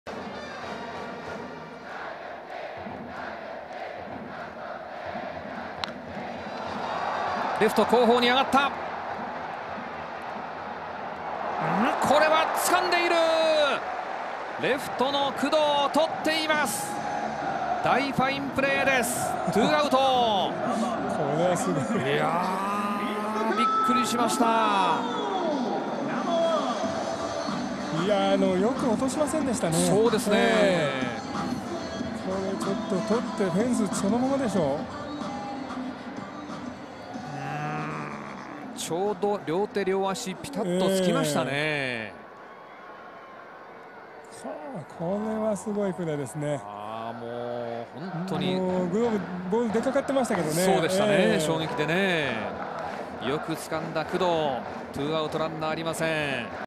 【フェンスに張り付く忍者捕球】工藤隆人がスーパーキャッチ
移籍してきた工藤隆人が懸命のプレーで存在感をアピール。フェンスに両手両足をつけるスーパーキャッチを見せた。(2011年7月18日 オリックス・バファローズ対千葉ロッテマリーンズ)